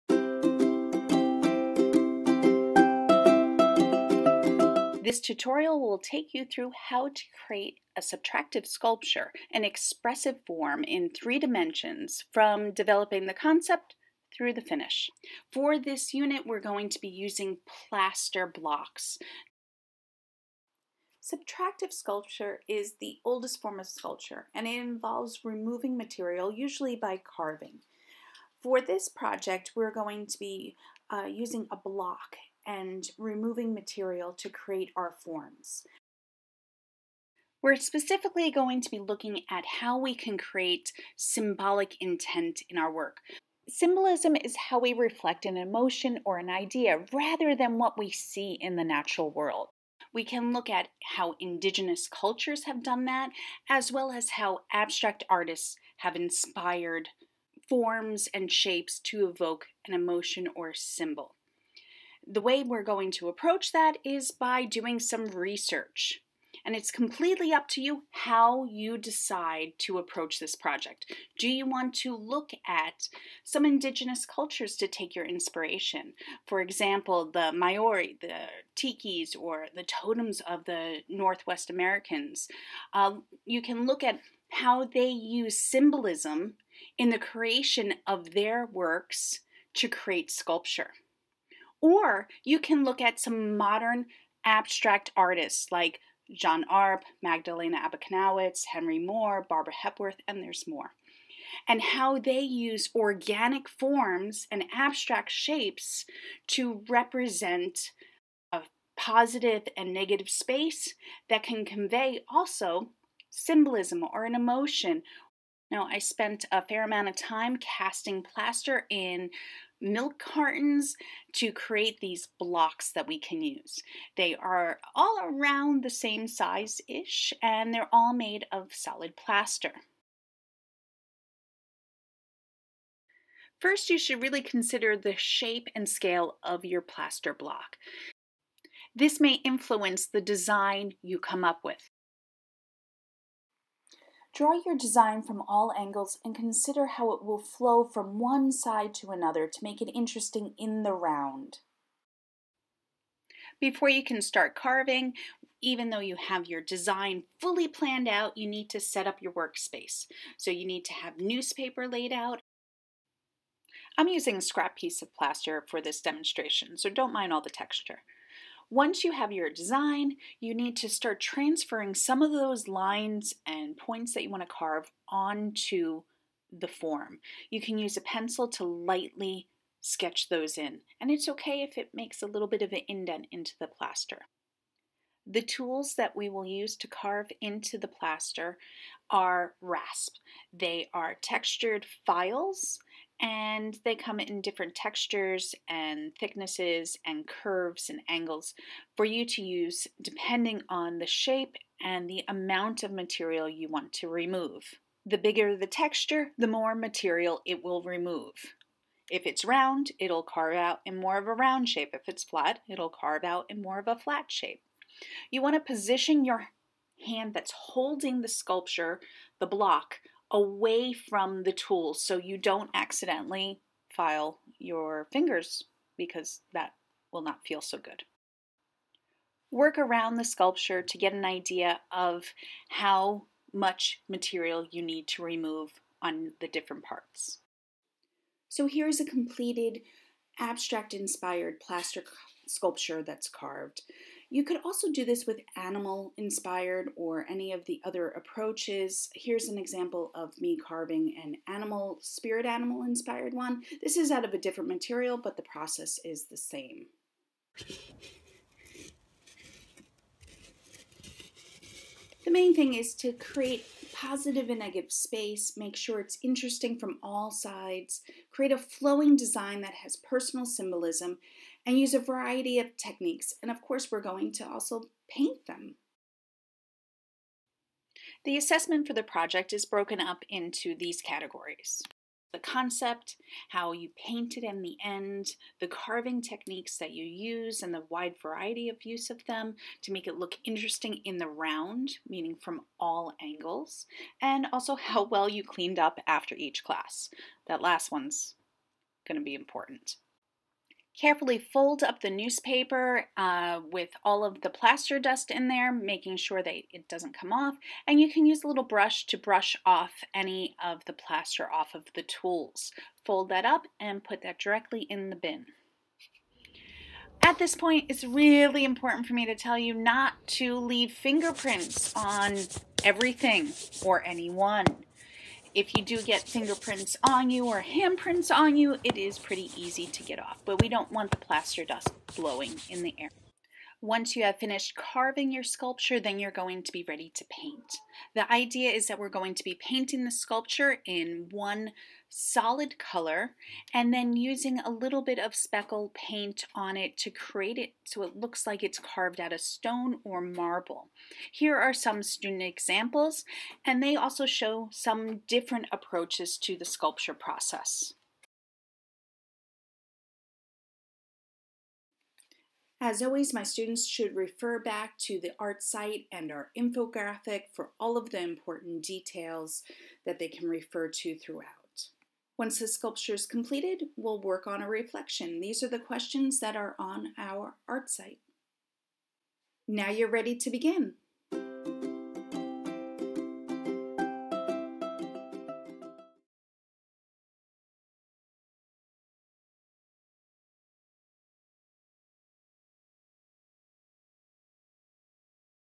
Subtractive Sculpture with Plaster
Middle school art demonstration:
Learn art techniques along with tips and tricks to make the process go smooth and easy.  More lessons are found below:

The first part of the demonstration of subtractive carving is inspired by how indigenous cultures use symbolism in their work (Tikis, Totems, etc..),  The goal is not to copy those indigenous culture's work but rather see how they use symbolism and apply it in relief and subtractive sculpture and create your own imaginative work that functions well in the round.
Some students opt for animals or expressive abstract work to inspire their designs.
See which tools can be used and some basic approaches for carving.

Jessica Russo Scherr- Fine Artist @bluelavaart